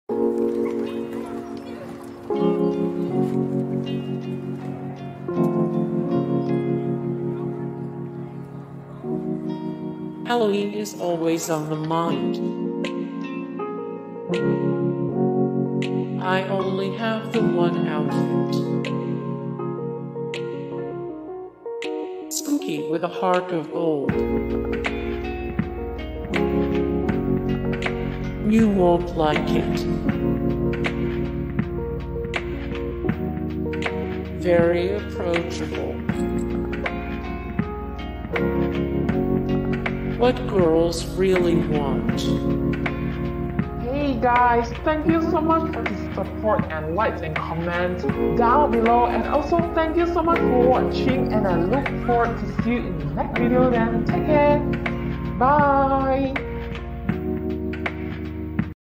20 Memes For World Goth Day To Prove It Wasn'T Just A Phase 🌠
It wasn't just a phase, mom.
Photo: user uploaded image
0:00 - Intro
0:00:08 - Halloween Is Always On The Mind
0:00:15 - I Only Have The One Outfit
0:00:22 - Spooky With A Heart Of Gold
0:00:29 - You Won't Like It
0:00:36 - Very Approachable
0:00:43 - What Girls Really Want